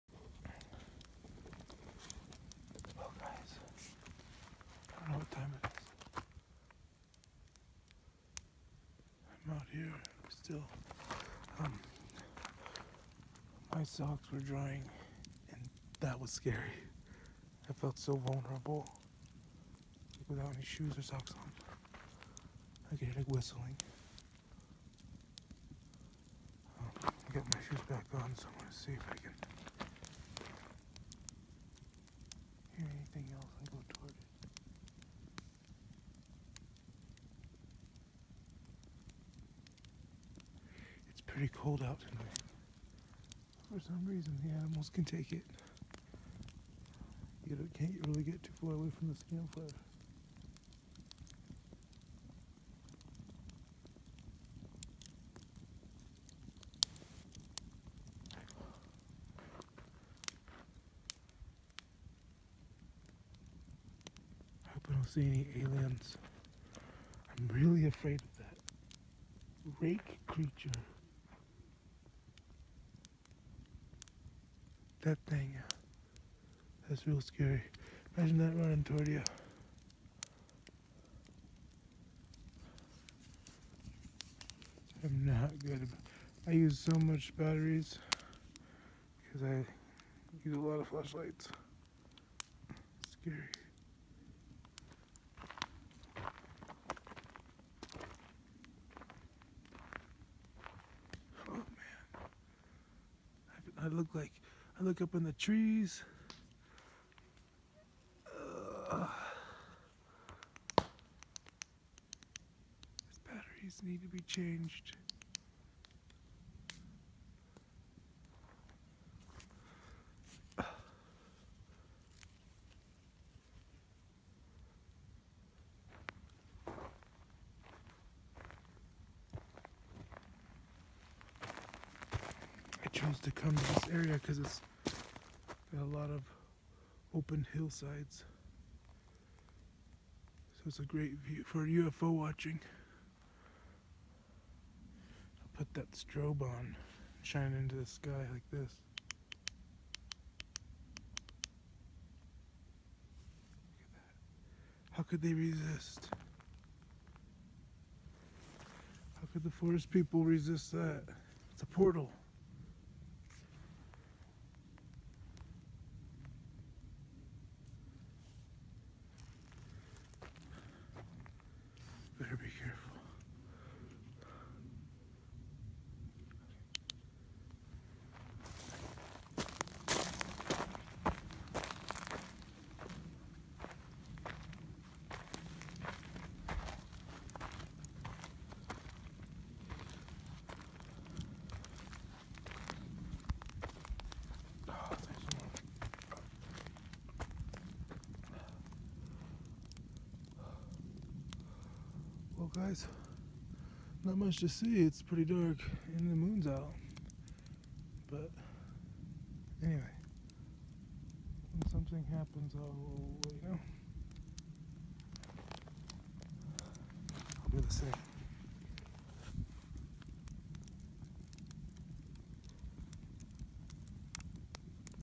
Midnight Monologues
Just sharing some thoughts while drying and keeping warm by the fire.